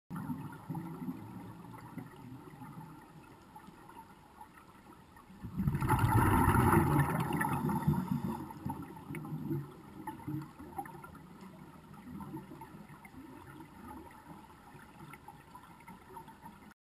underwater stock footage of Black rock fish in between kelp strands
Underwater stock footage (low res sample of High Definition footage) of Black Rock fish in between kelp strands over reef.

1920 x 1080 30fps
File ref: FITWRFBLKJ2012001